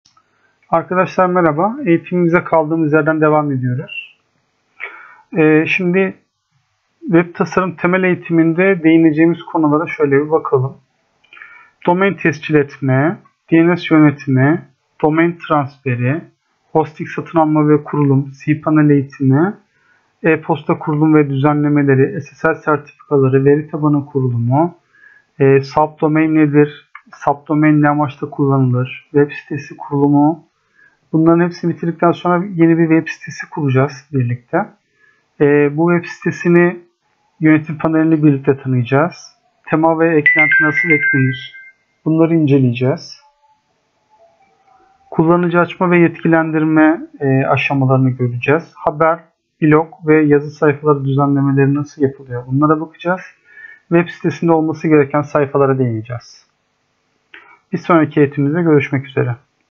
Noisoft Akademi web tasarım temel eğitimi 02
Noisoft Akademi web tasarım temel eğitimimizde, web tasarımın temellerinden başlayarak kendi web sitesini kurmak isteyen amatörlerden bu işi meslek olarak yapmak isteyen profesyonellere kadar herkes için web tasarım eğitimi veriyoruz. Temel seviye, Orta seviye ve İleri seviye olmak üzere 3 farklı eğitim vermekteyiz. Temel seviyede bir web sitesi için gereken herşeyi sıfırdan öğrenecek, Kendinize ait bir Domain adresi satın alıp, Web sitenizi yükleyeceğiniz bir hostinge nasıl yönlendireceğinizden başlayarak, birkaç saatlik temel eğitim bilgilerinden sonra, sıfırdan bir web sitesini birlikte  oluşturacağız. Oluşturmuş olduğumuz web sitesini ziyaret ederek canlı olarak inceleyebileceksiniz ve kendiniz de aynı şekilde sıfırdan ve tamamen ücretsiz bir web sitesi oluşturabileceksiniz. Orta ve ileri seviye eğitimlerimiz de ise öğrendiklerimiz üzerine ilave bilgiler öğrenerek bu işin uzmanlığını yapabilecek seviyeye erişebileceksiniz. Eğitimimizde görüşmek üzere...